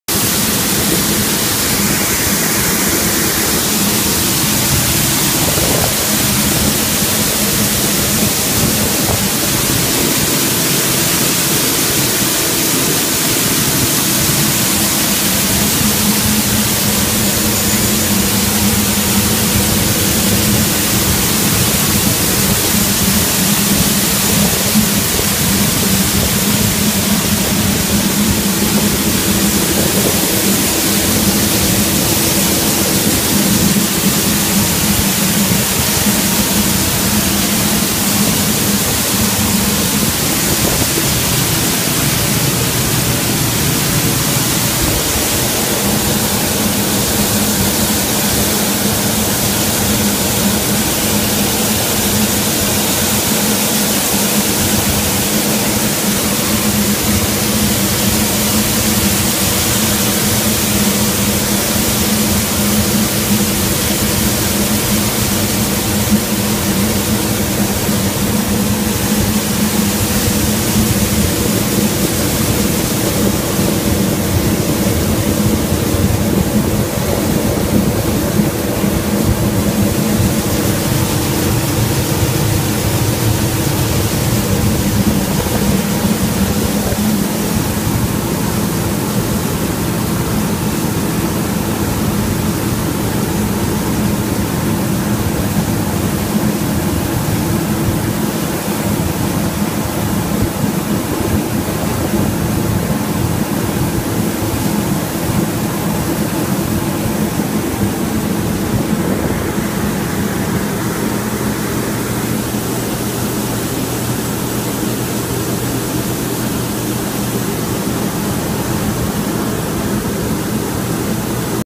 穀物用循環型乾燥機 静岡製機 PCS-13F 内部
籾摺りをするために排出中の乾燥機内部です。排出しきって空っぽになるところがわかります。